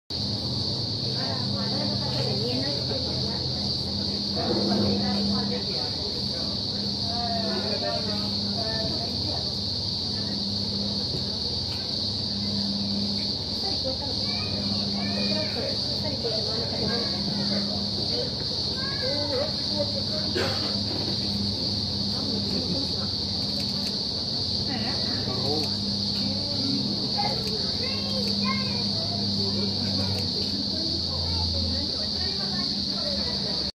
Blue Mountains Cable Car - Blue Mountains NSW
Another beautiful Day to be in the Blue Mountains with Sydney Private Day Tours! Waiting for the Cable Car to arrive with the noise of lots of Cicadas in the background!  To book a Private Tour of the Blue Mountains